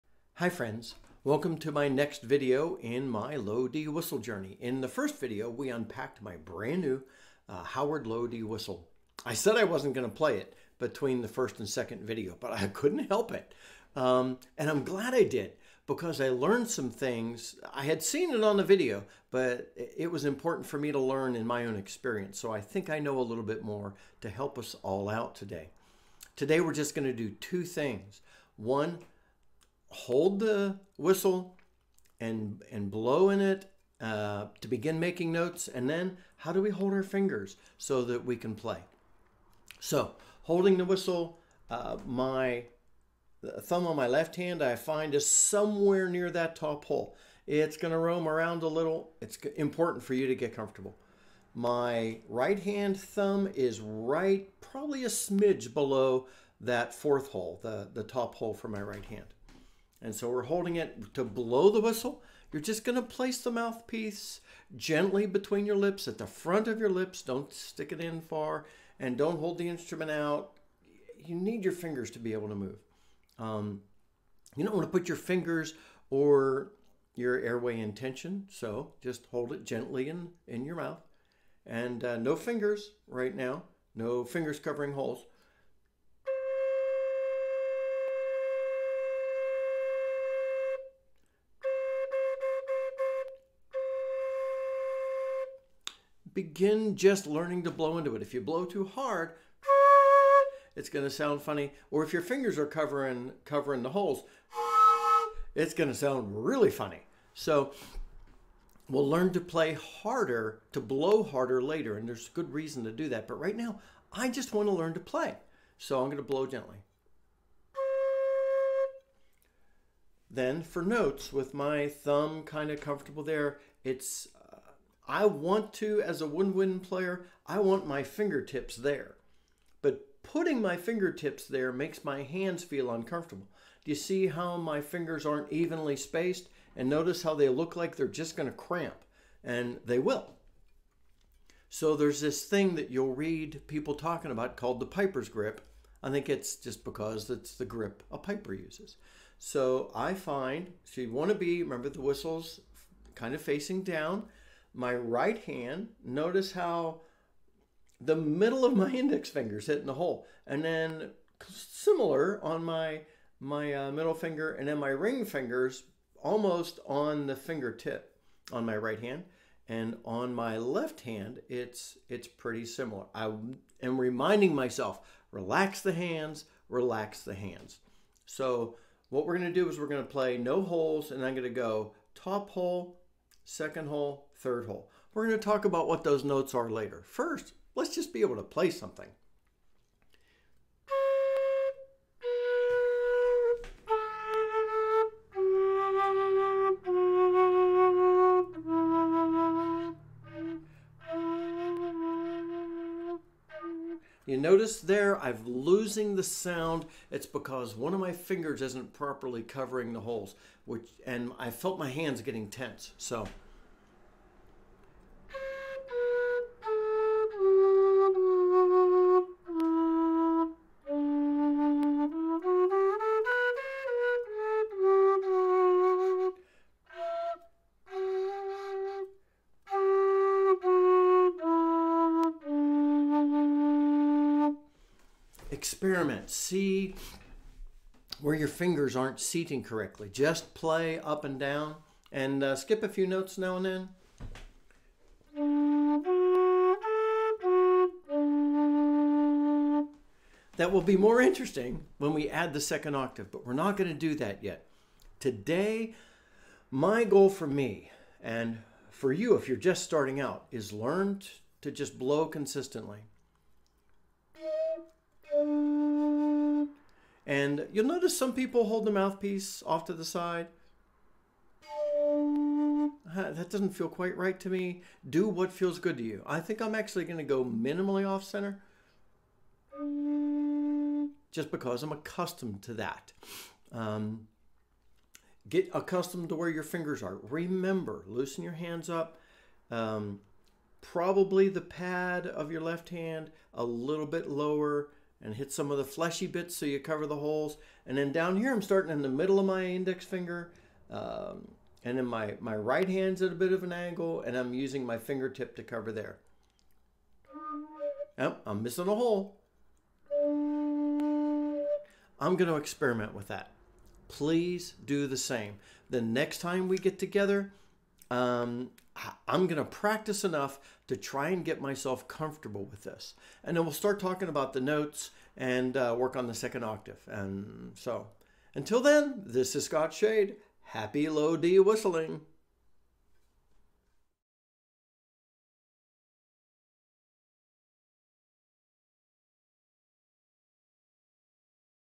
Getting Started Playing a Low D Whistle
In today's video we begin the process of learning to play the low D whistle. I am using my Howard Low D.

We learn how to hold the whistle, placing our fingers over the holes, and playing our first notes.

From here we play, play, play.